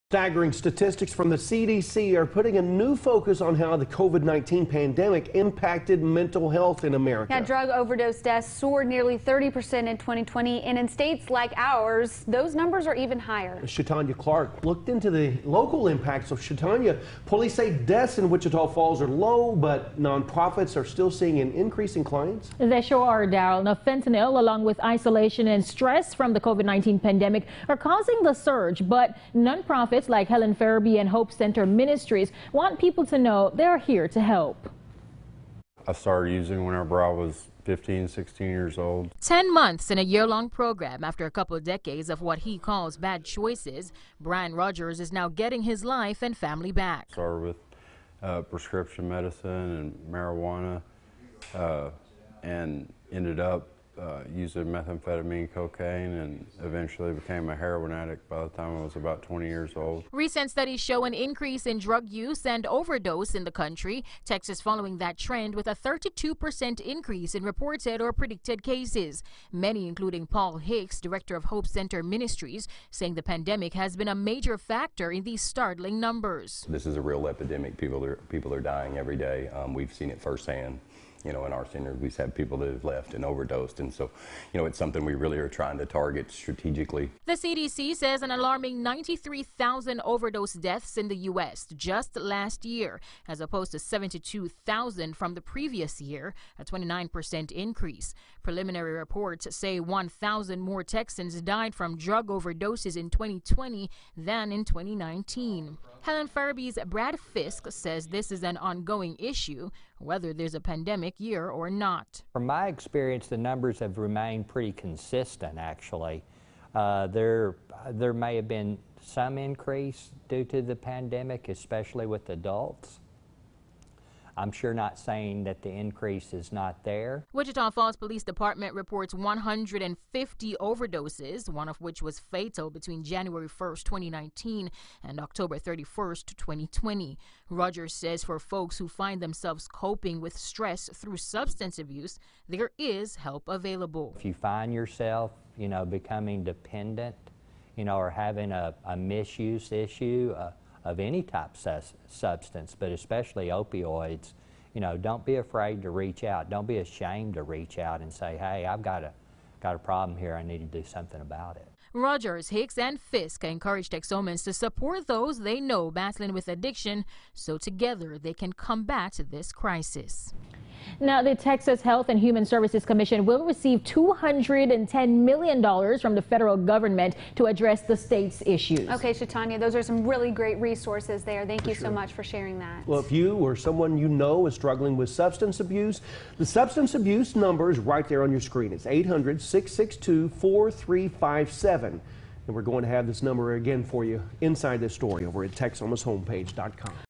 Texas sees climb in drug overdose deaths, Wichita Co. nonprofits work to combat local problem
Staggering statistics from the Centers for Disease Control and Prevention are putting a new focus on how the COVID-19 pandemic impacted mental health in America.